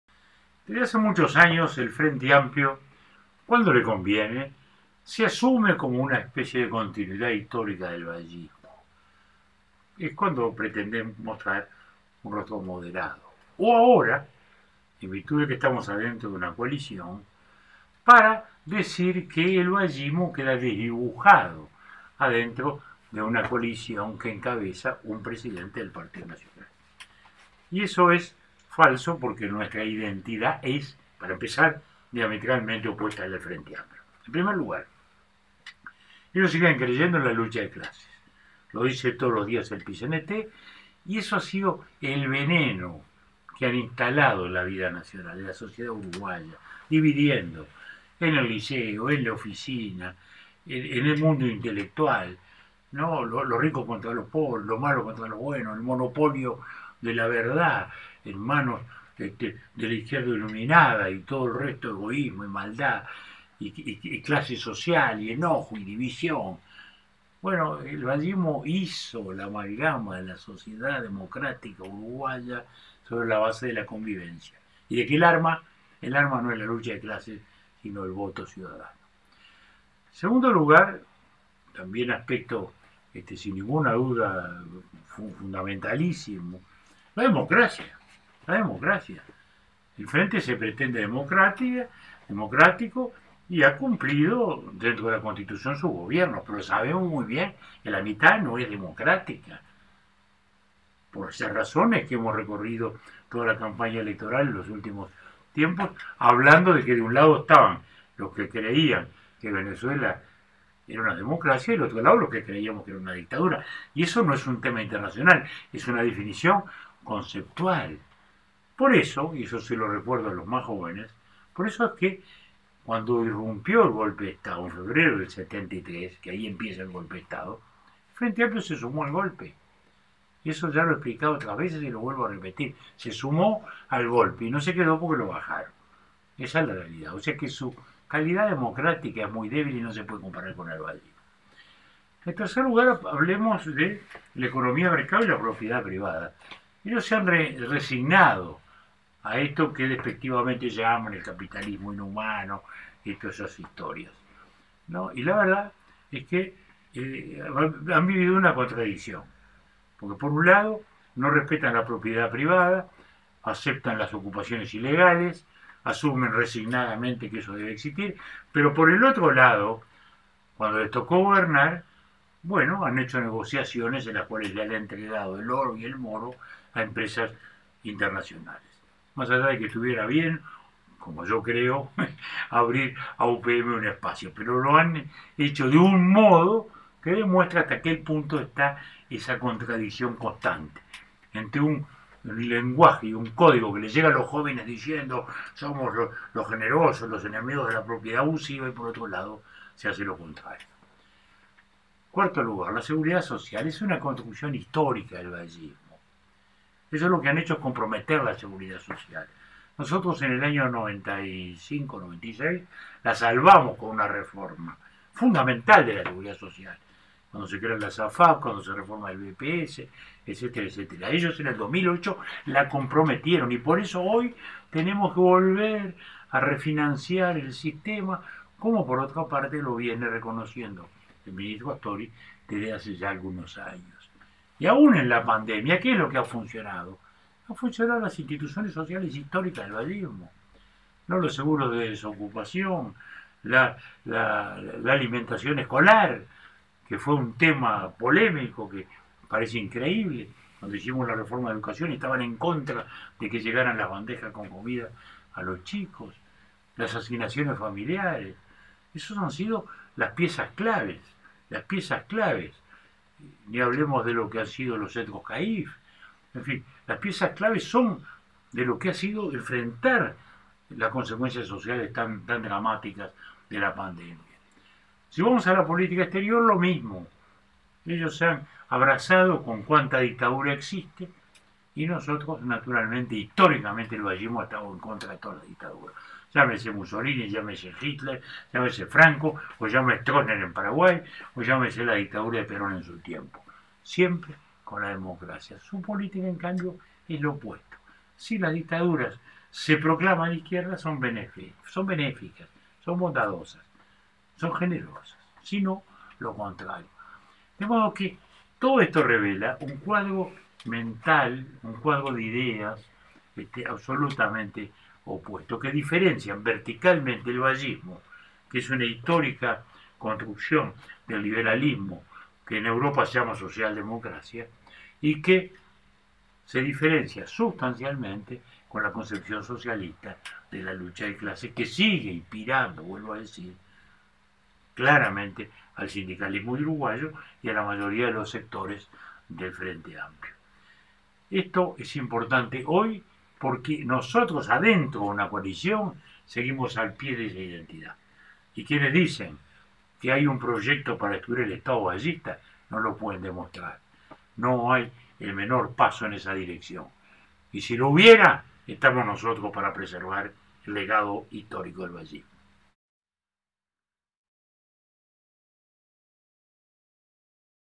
El Batllismo solo puede estar en el Partido Colorado
Un breve video en el que el Secreterio General Julio María Sanguinetti repasa por qué el Batllismo solo puede estar en el Partido Colorado y nunca en el Frente Amplio.